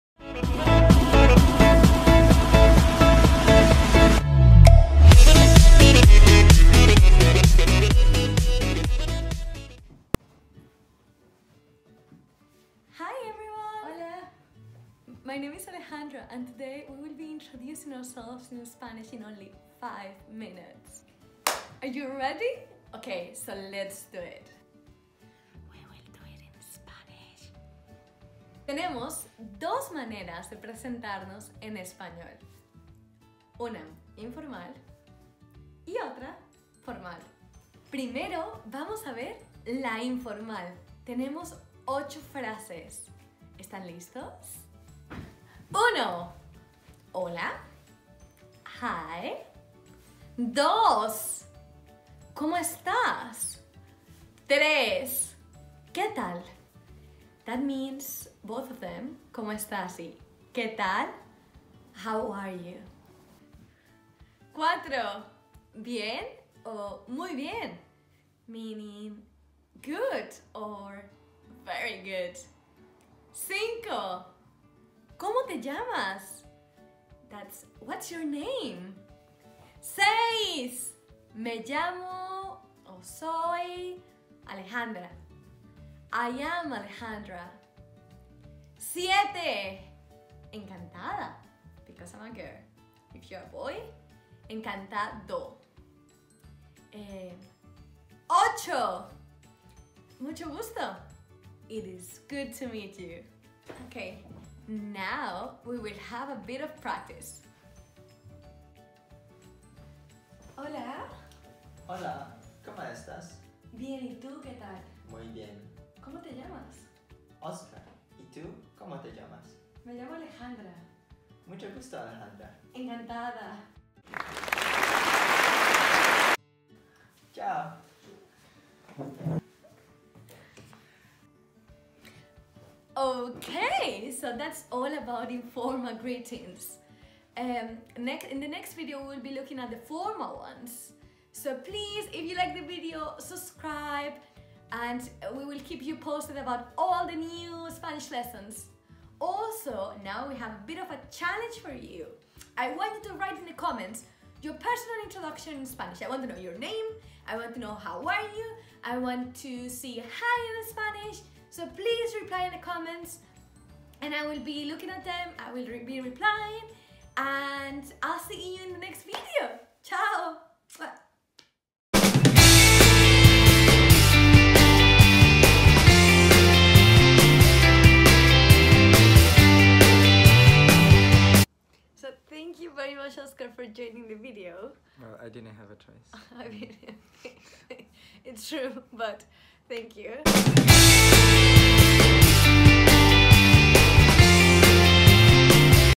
Spanish for Beginners 1: Informal Greetings
Spanish for beginners - Lesson 1.

Learn all about informal greetings in Spanish! In this lesson, we will teach you 8 useful phrases for having an informal conversation. Once you watch the video feel free to answer our challenge and tell us your personal introduction in Spanish!

The personal introduction has to include:

1. A Greeting
2. Nombre
3. ¿Cómo estás?

We will be replying your comments in Spanish!